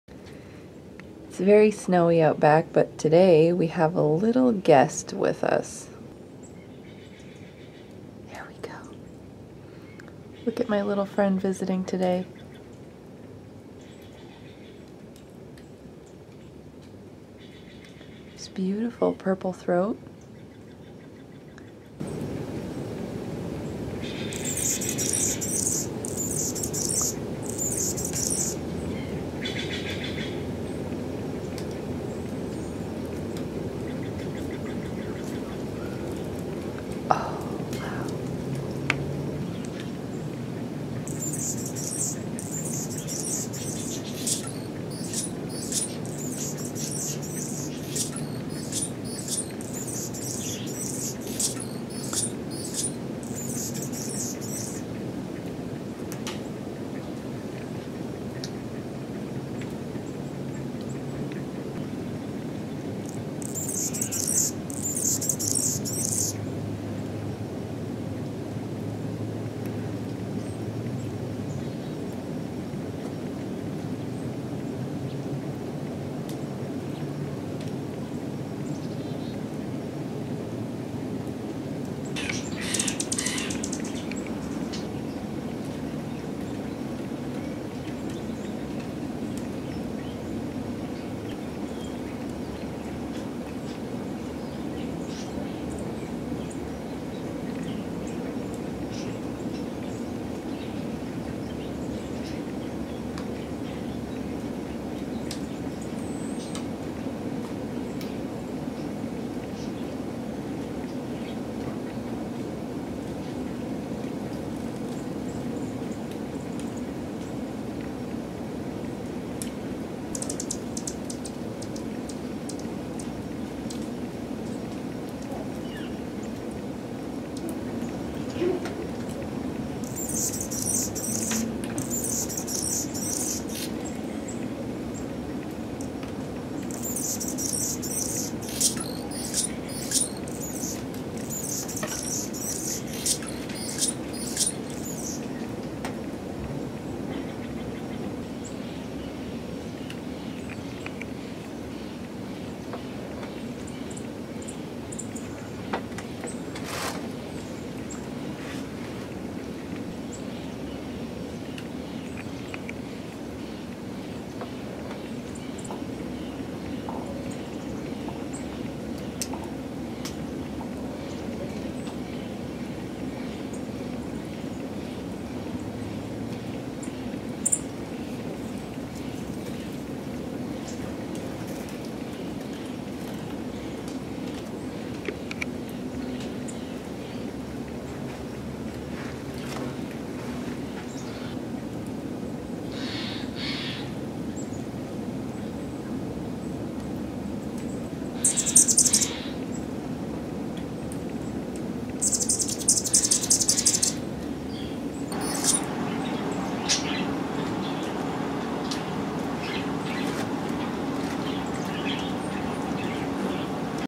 Anna's Hummingbird song - Pacific Northwest 2017
This little bird hung around outside my porch for a few hours today, so I wanted to film him. He shit a lot.